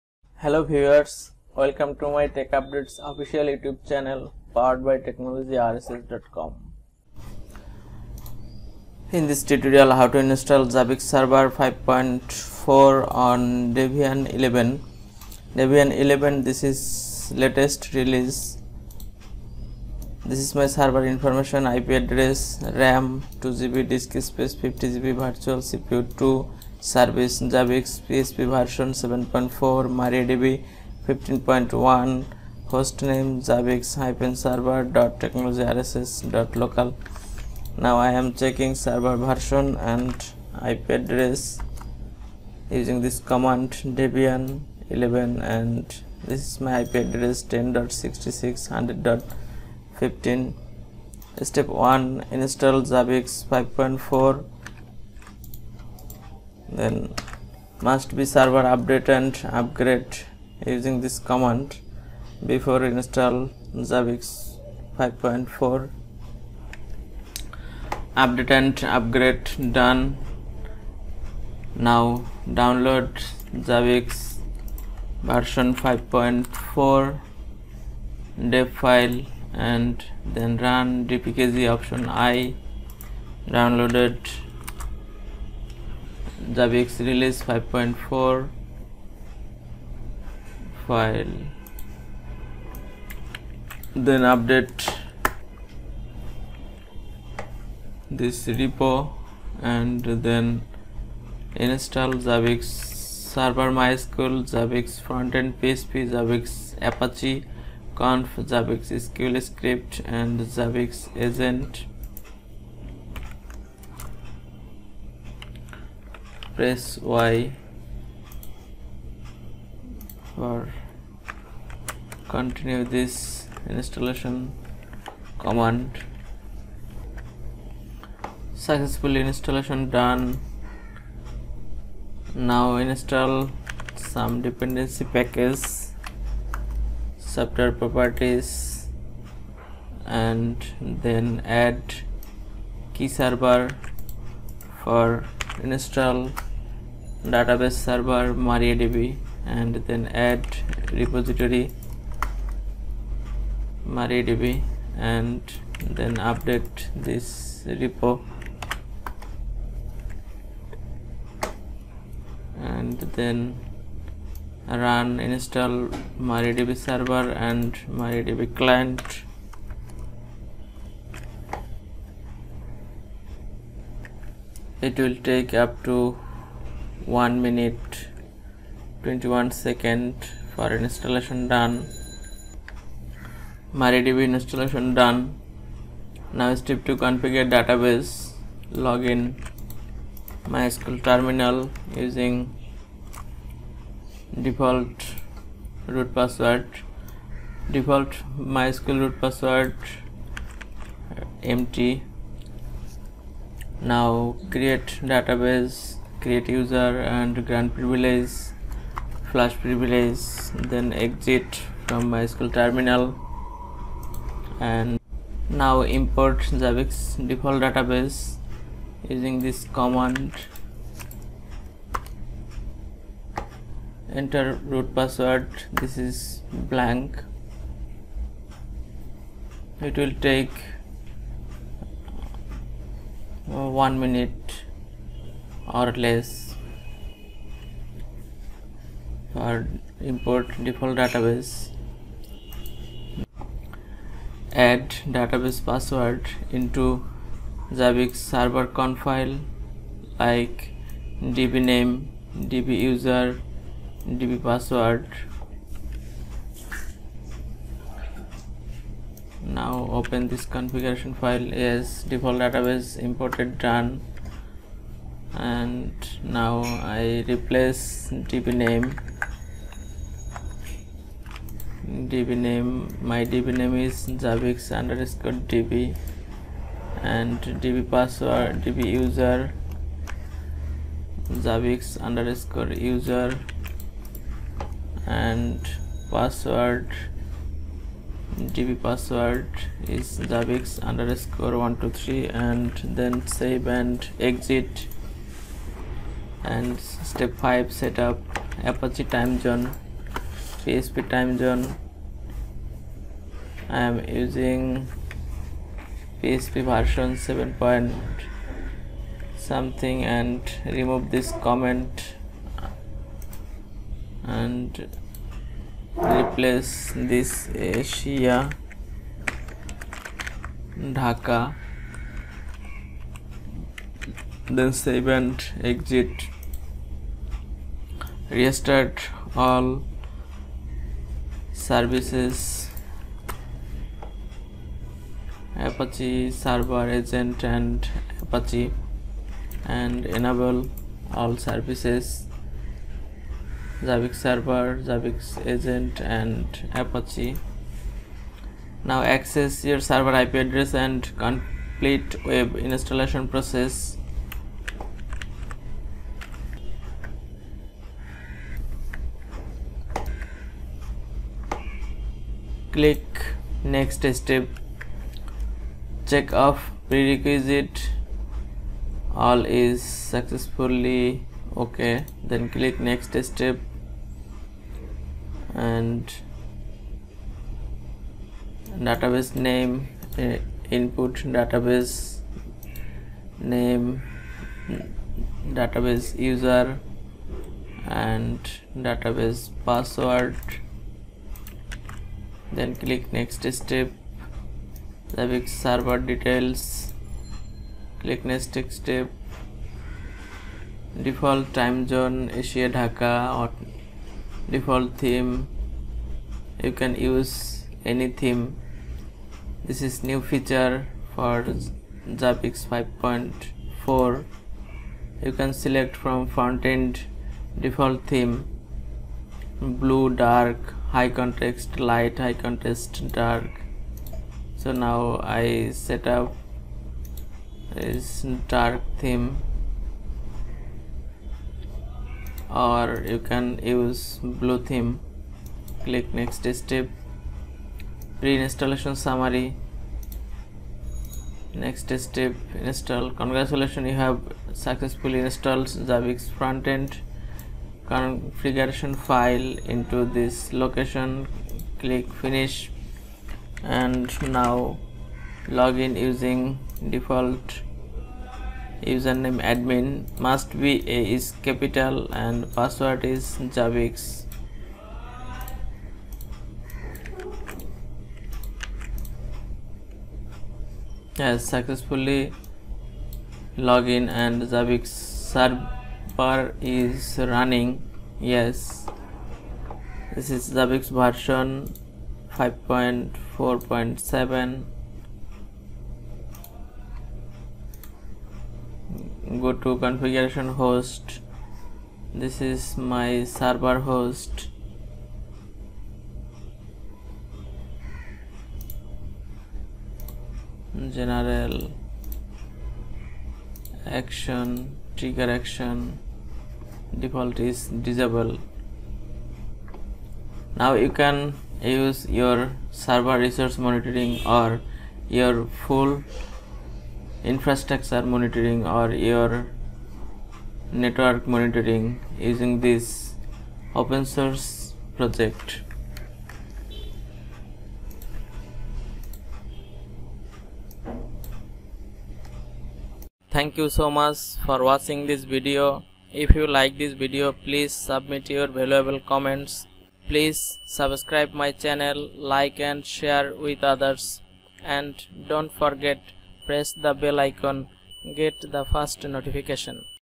How To Install Zabbix Server 5.4 On Debian 11
Now I want to share How To Install Zabbix Server 5.4 On Debian 11

Thanks.